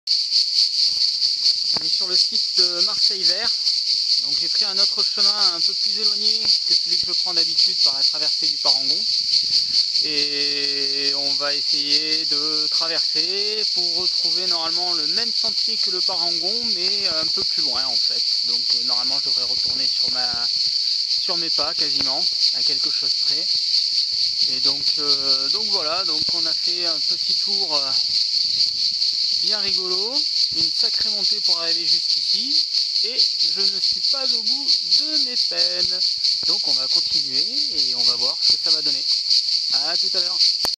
2012-07-26 - Première vidéo
Première video juste sur le promontoire rocheux aux dessus de la Grotte Roland